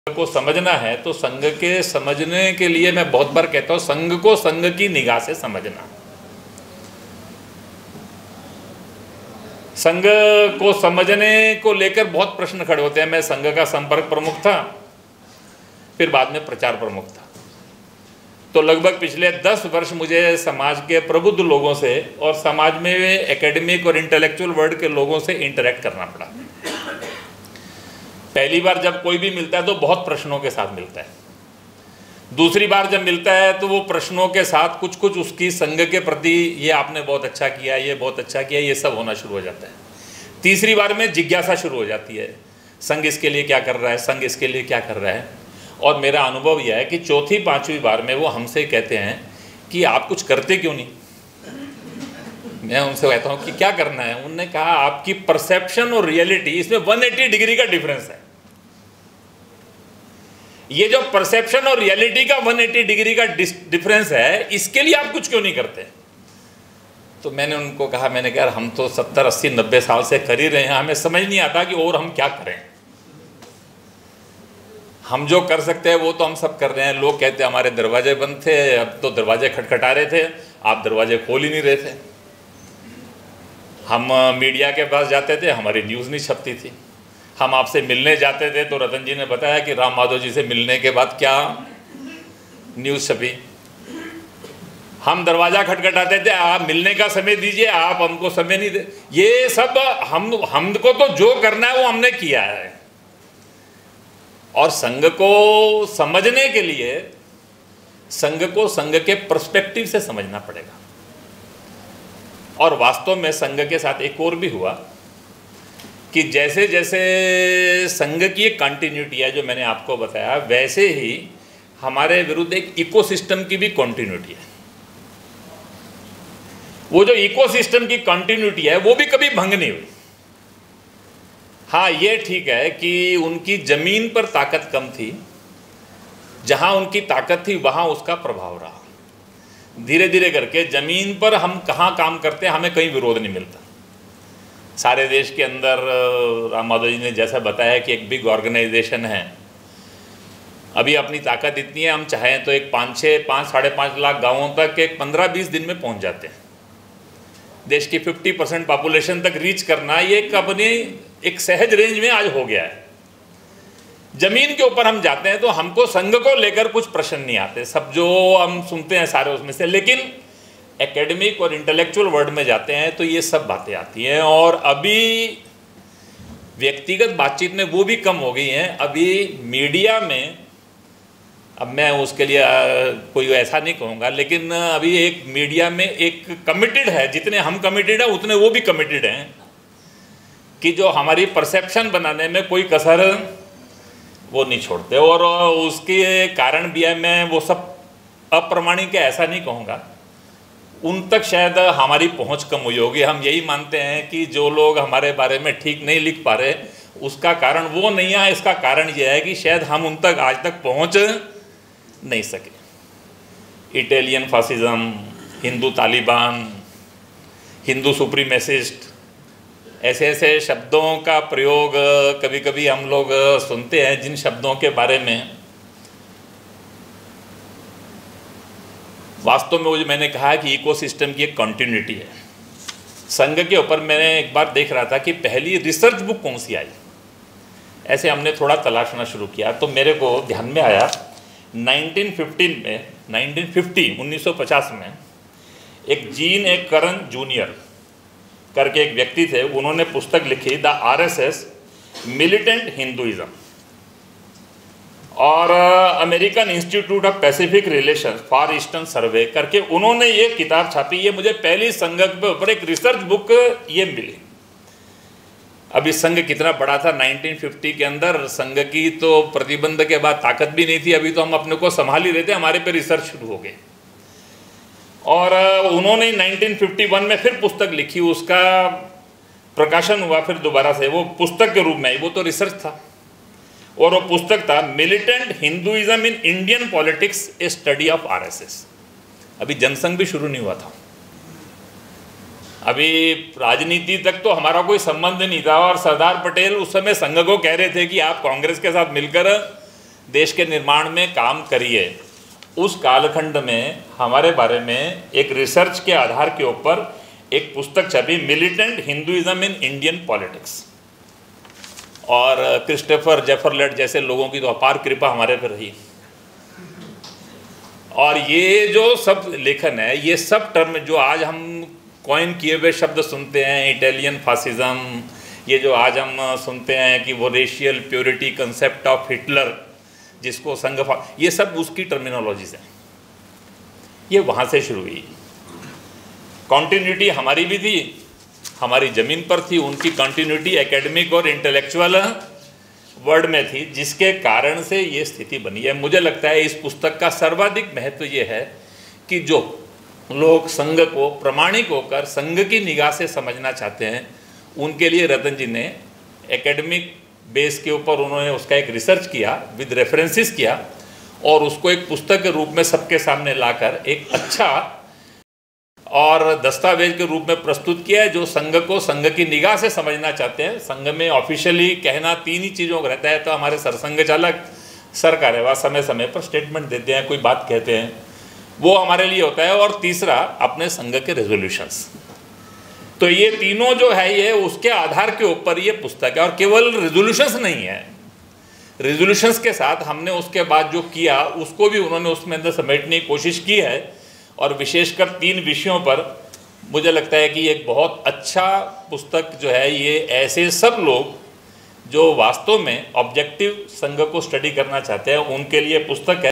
संघ को समझना है तो संघ को संघ की निगाह से समझना - अरुण कुमार
राष्ट्रीय स्वयंसेवक संघ को समझना है तो संघ को संघ की निगाह से समझना

पहली बार जब कोई हमसे मिलता है तो बहुत प्रश्नों के साथ मिलता है। दूसरी बार उनके प्रश्नों के साथ आपने ये अच्छा किया, वो अच्छा किया, यह सब होना शुरू हो जाता है। तीसरी बार उनमे जिज्ञासा शुरू हो जाती है कि संघ इसके बारे में क्या कर रहा है। चौथी-पांचवी बार में वह हमसे कहते हैं कि आप (संघ) कुछ करते क्यों नहीं, संघ के परसेप्शन एवं रियलिटी में 180 डिग्री का अंतर है। - श्री अरुण कुमार जी, सह सरकार्यवाह,  राष्ट्रीय स्वयंसेवक संघ